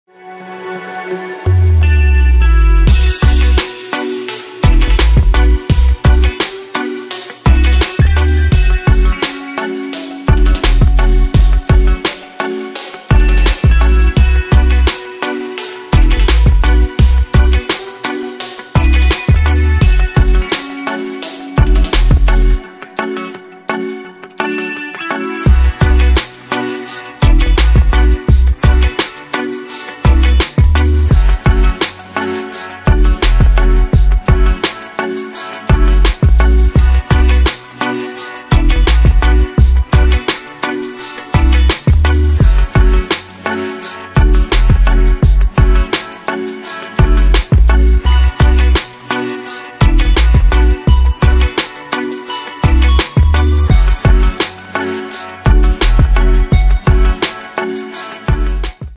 Beat Reggae-ish
Dog day afternoon, cu un nou beat/instrumental mai pe chill.
De vanzare, doar cu optiune de licenta exclusiva de master (nimeni nu il mai poate cumpara dupa prima vanzare). Detalii
Toate drepturile rezervate. DAW.RO